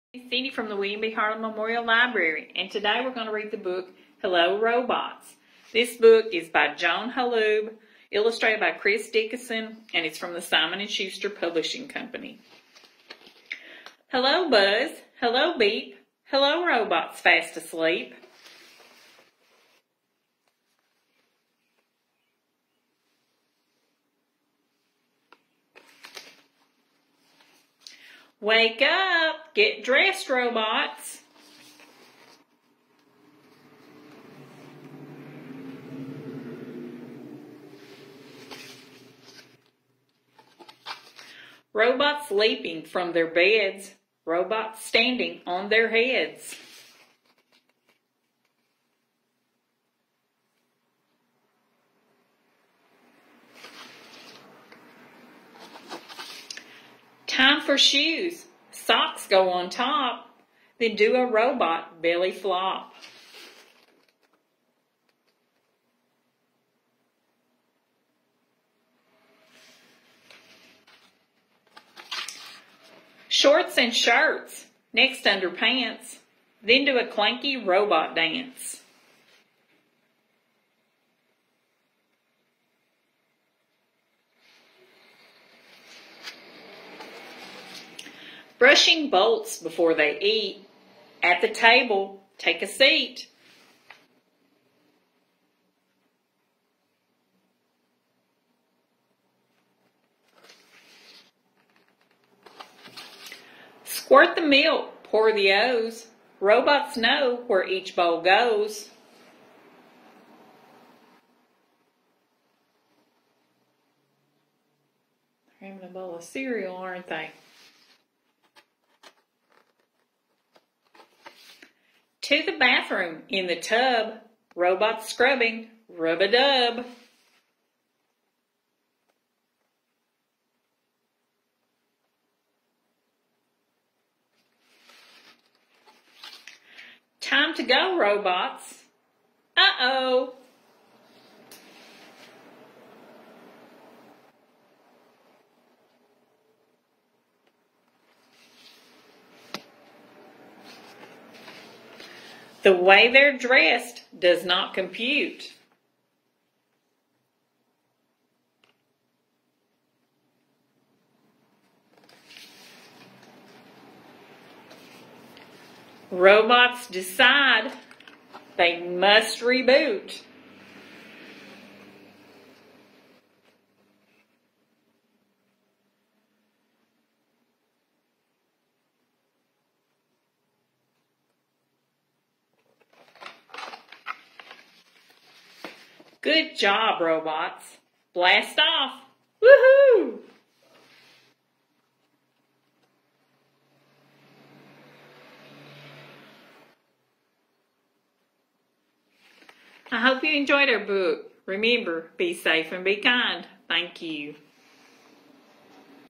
Afterschool Story with WBHML: "Hello Robots!" with Ms. Cindy
In this Afterschool Story with WBHML, Ms. Cindy reads "Hello Robots!" written by Joan Holub and illustrated by Chris Dickas with permission of Simon & Schuster Publishing.